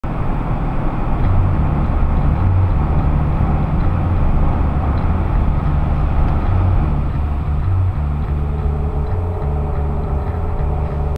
WTF! Pickup dump truck is raised on highway (2014-11-05)
Pickup truck with dump-truck bed is raised while driving on the highway!  Hopefully he didn't go under any low bridges!